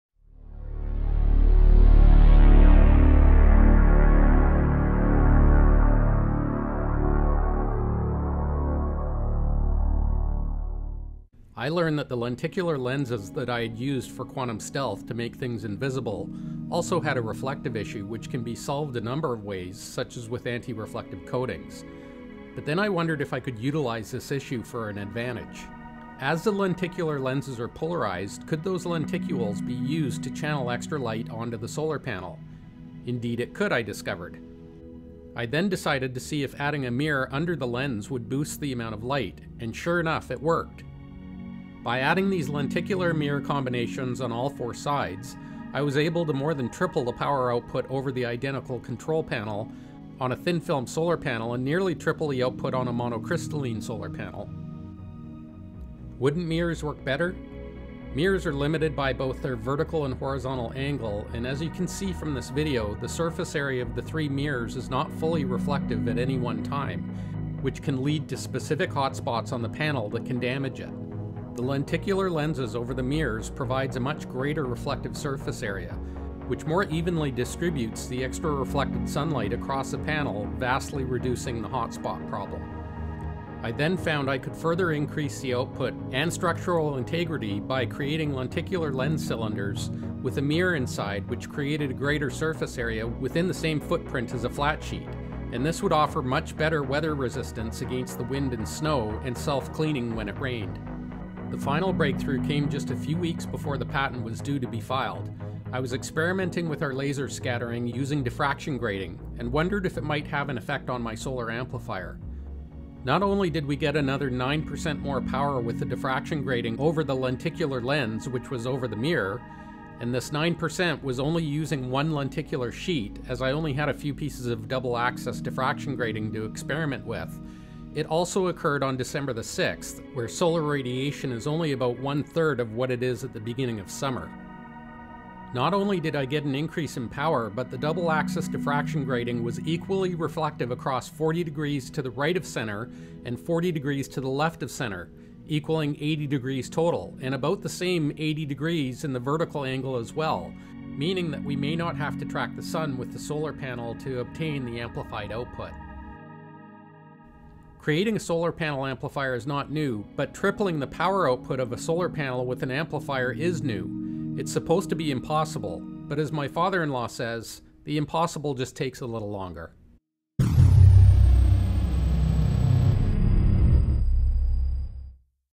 Hyperstealth patent pending Solar Panel Amplifier
Hyperstealth's patent pending Solar Panel Amplifier has been demonstrated to more than triple thin film solar panel output and nearly triple the common monocrystalline solar panels.